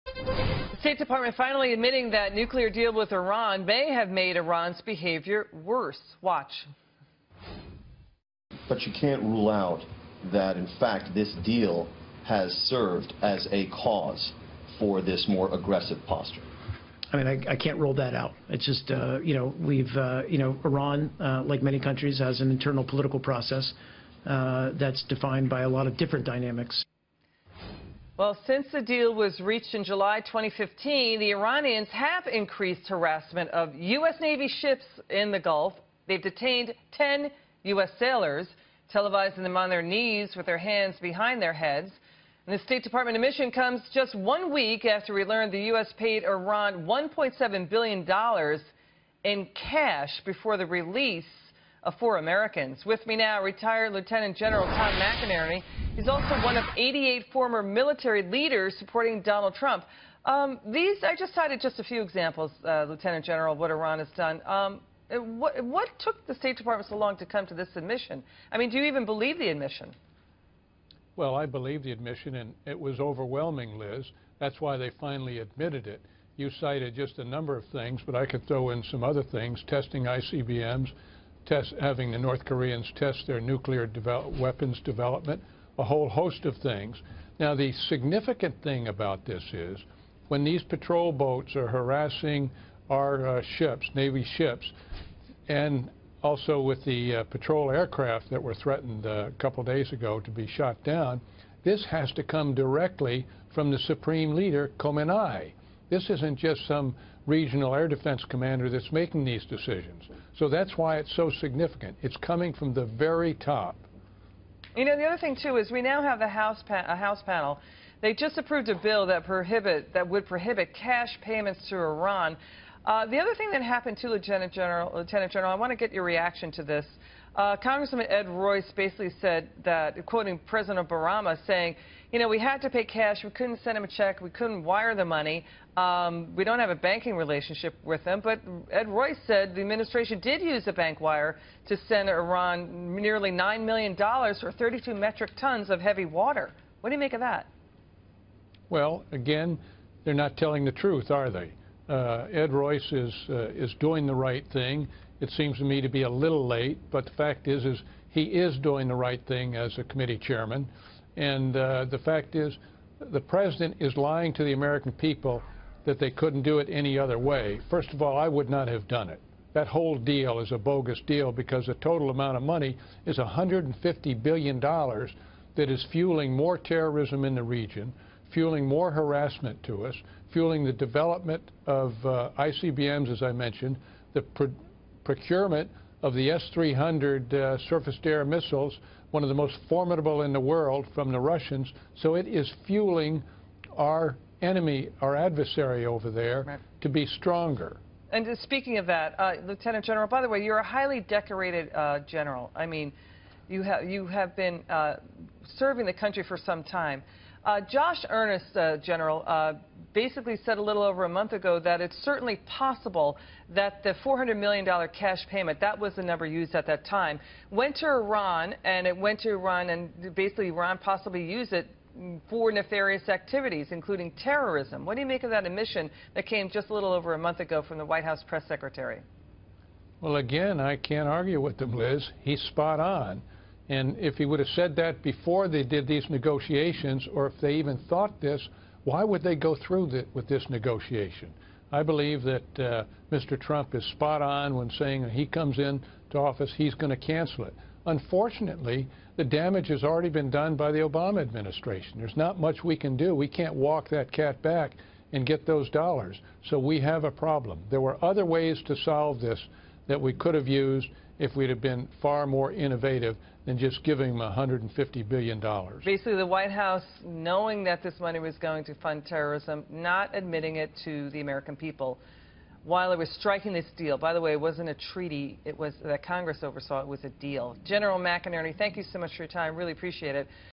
Did the nuclear deal make Iran’s behavior worse?
(Ret.) Lt. Gen. Thomas McInerney on the State Department’s comments about the Iran nuclear deal.